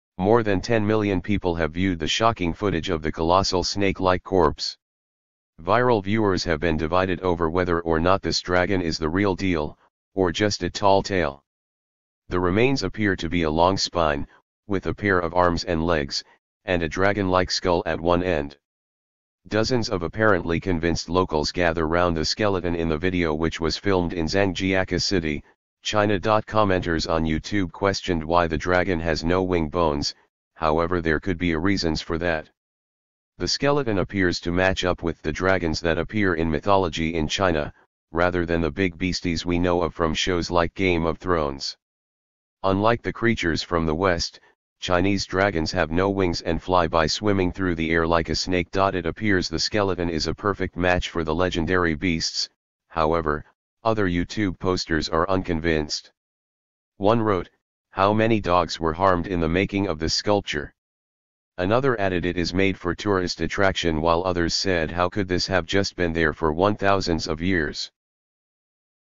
Mystery as 60ft long ‘DRAGON skeleton’ suddenly appears – and authorities are BAFFLED
More than 10 million people have viewed the shocking footage of the colossal snake-like corpse.

Viral viewers have been divided over whether or not this dragon is the real deal, or just a tall tale.

The remains appear to be a long spine, with a pair of arms and legs, and a dragon-like skull at one end.

Dozens of apparently convinced locals gather round the skeleton in the video which was filmed in Zhangjiakou City, China.Commenters on YouTube questioned why the dragon has "no wing bones" – however there could be a reasons for that.

The skeleton appears to match up with the dragons that appear in mythology in China – rather than the big beasties we know of from shows like Game of Thrones.

Unlike the creatures from the West, Chinese dragons have no wings and fly by “swimming” through the air like a snake.It appears the skeleton is a perfect match for the legendary beasts – however, other YouTube posters are unconvinced.

One wrote: "How many dogs were harmed in the making of this sculpture?"

Another added "It is made for tourist attraction" while others said "how could this have just been there for 1000s of years?".